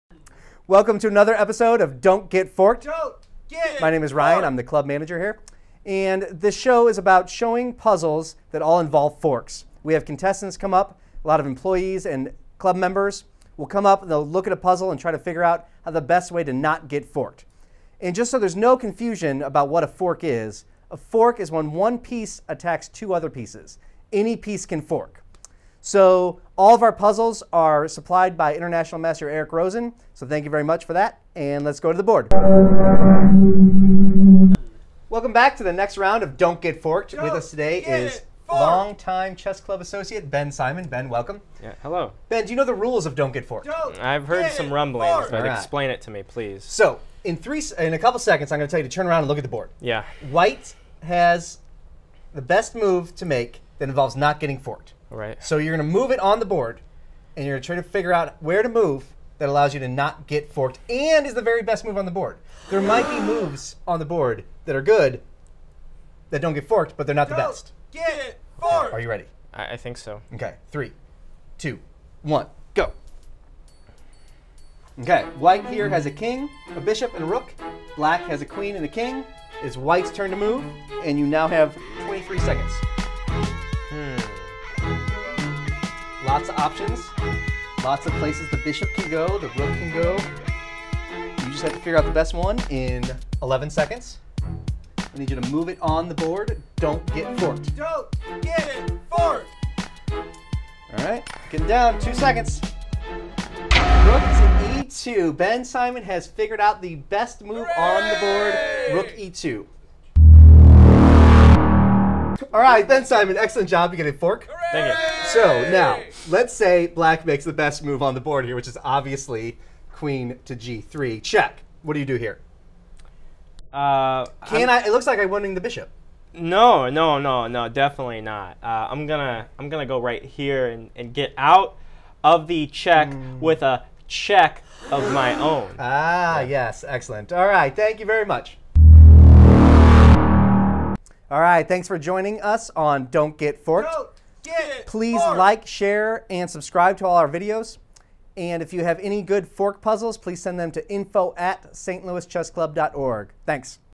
Don't Get Forked! Episode 6: Ben Simon
From the visionary creator of Blunders comes a brand new video series that will dazzle, amaze and confuse you.

Prepare to enter a world of cutlery so devastating that you won’t want to go into the kitchen for weeks.

Witness a world where average Chess players are given puzzles just above their ratings. . .